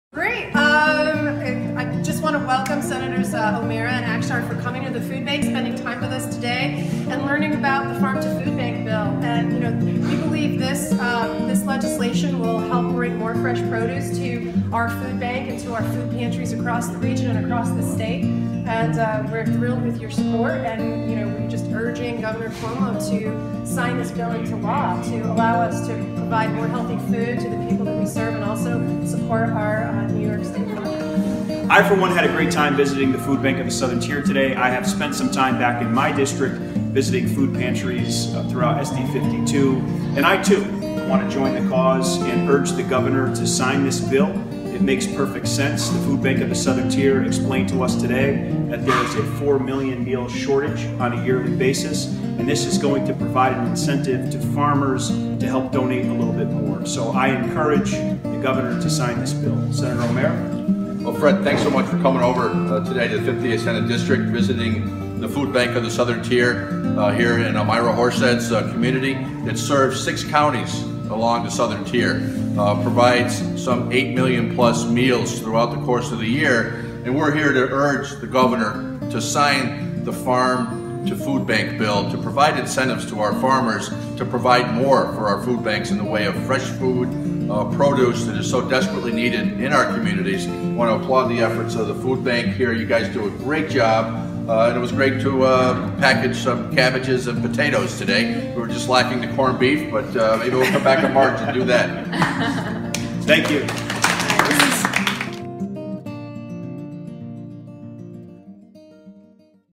Touring the Food Bank of the Southern Tier
Thank you to the Food Bank of the Southern Tier for educating myself and New York State Senator Tom O'Mara on all the good work that they do for those in need across the communities we represent. We're urging the governor to sign our Farm-to-Food Bank legislation to help support farmers donating their food to those in need.